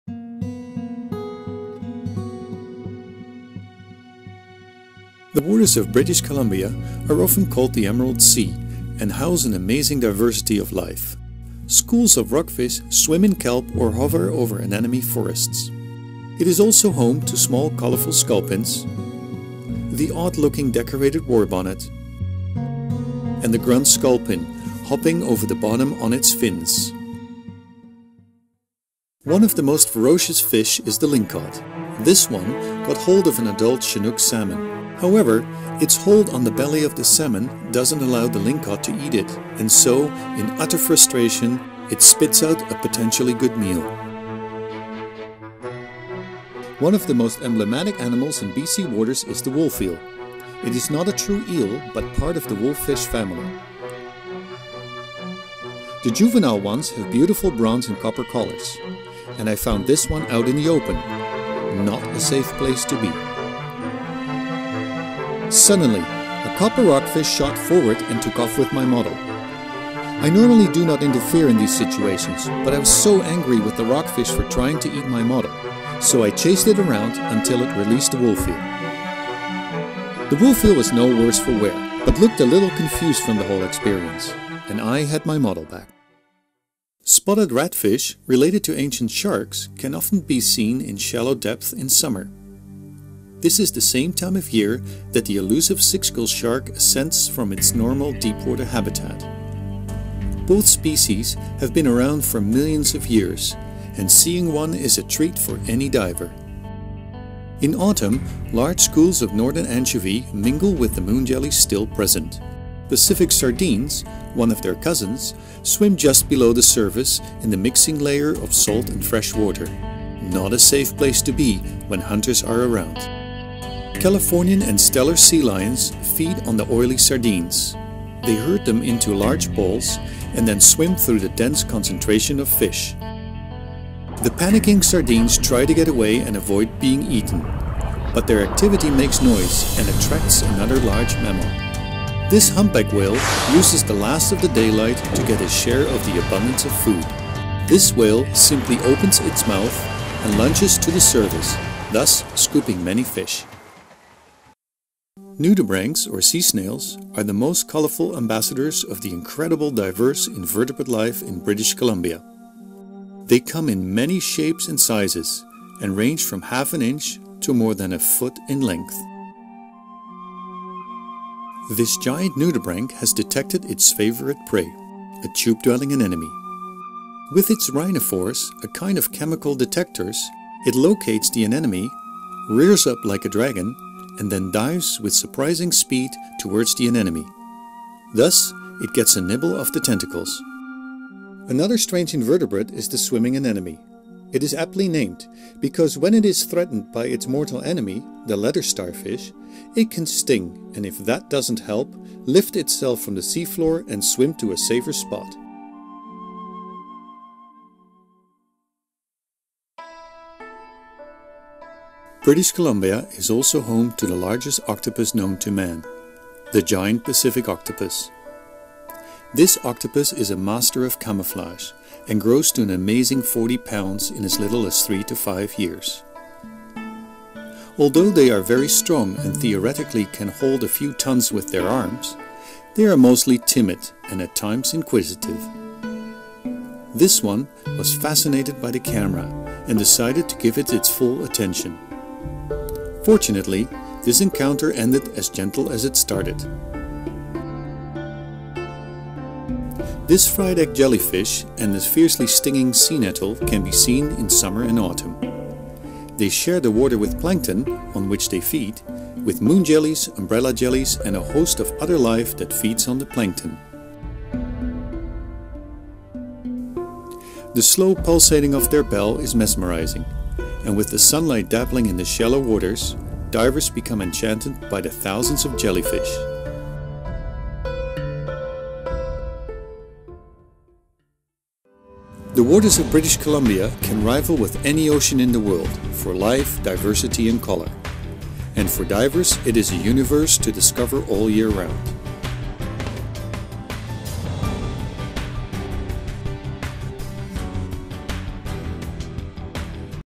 Rhapsody in Green, immersion in BC's marine biodiversity
This 5 minutes video was screened at the San Diego Undersea Film Exhibition in Oct. 2014. It takes you through a selection of the marine biodiversity in the waters of British Columbia.